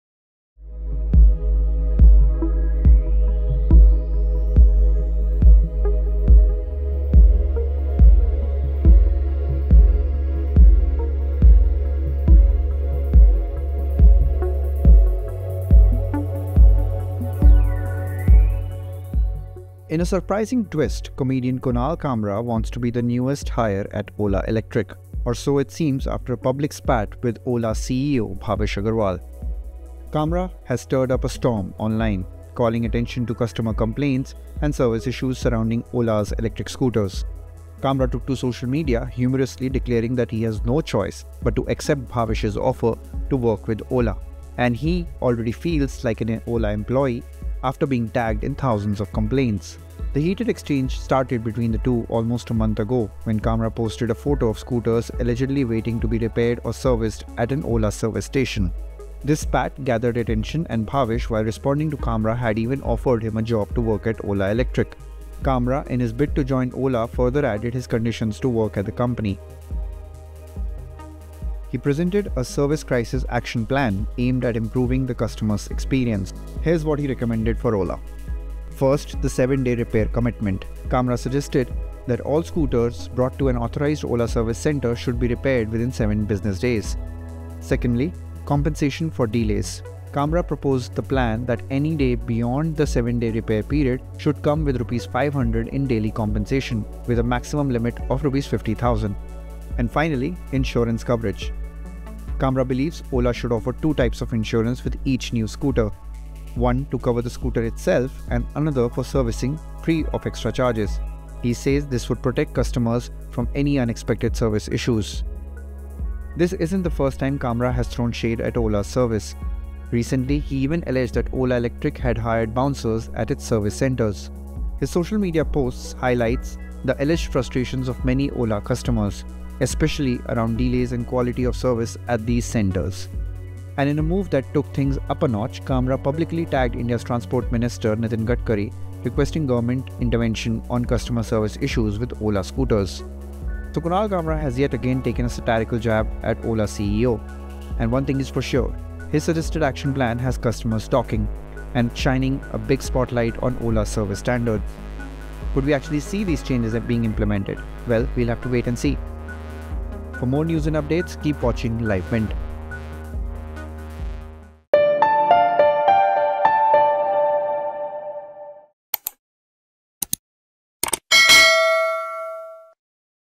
Ola CEO vs Kunal Kamra: Comedian 'Accepts' Offer To Join Ola Electric But Here Are His Conditions…
In a surprising twist, comedian Kunal Kamra wants to be the newest ‘hire’ at Ola Electric — or so it seems after a public spat with Ola’s CEO, Bhavish Aggarwal. Kamra has stirred up a storm online, calling attention to customer complaints and service issues surrounding Ola’s electric scooters.

Kamra, in his bid to join Ola further added his conditions to work at the company.